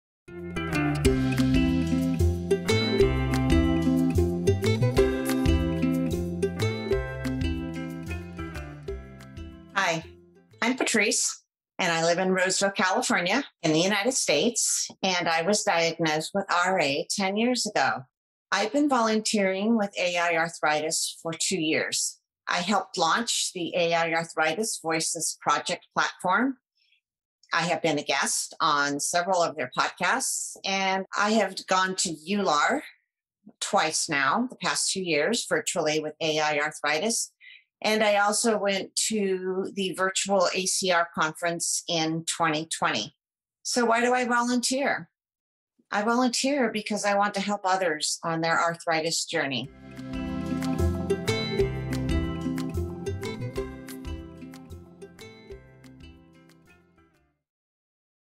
AiArthritis Volunteer Interviews: Patrice Johnson
Meet Patrice! You may know her from our Talk Show, and she is a regular participant in our “Go with Us” scientific conference program. Diagnosed with RA, she is driven by her compassion to help others on their journey, and we are so glad to have her on our side.

Volunteers mean the world to us! They are at the heart of everything we do.
to sign up and learn how YOU can change the world from your sofa!

Learn all about volunteering with AiArthritis in this Voices 360 Talk Show episode